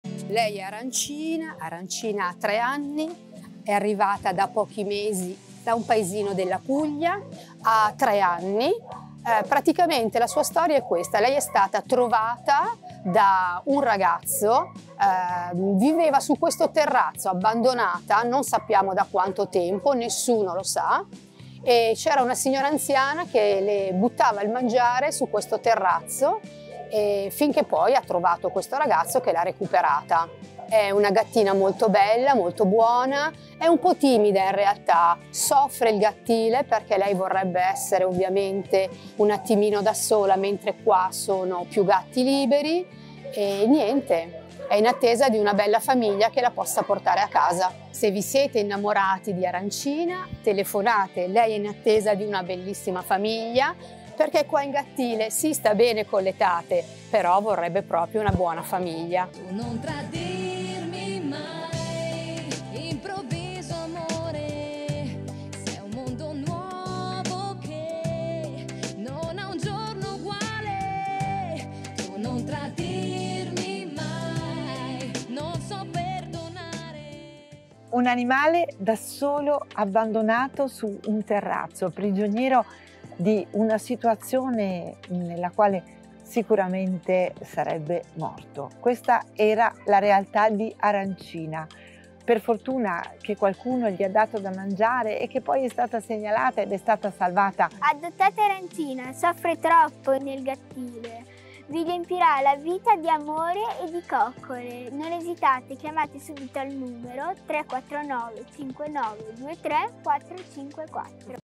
PUNTATA DEL 4 OTTOBRE, ADOZIONE 3: ARANCINA
ARANCINA è una dolce micia di tre anni arrivata da un paesino della Puglia. È stata trovata da un ragazzo abbandonata su un terrazzo. Buona e timida soffre il gattile! Adottatela chiamando Asilo del Cane - Paderno Dugnano (MI) 📲 349/5923454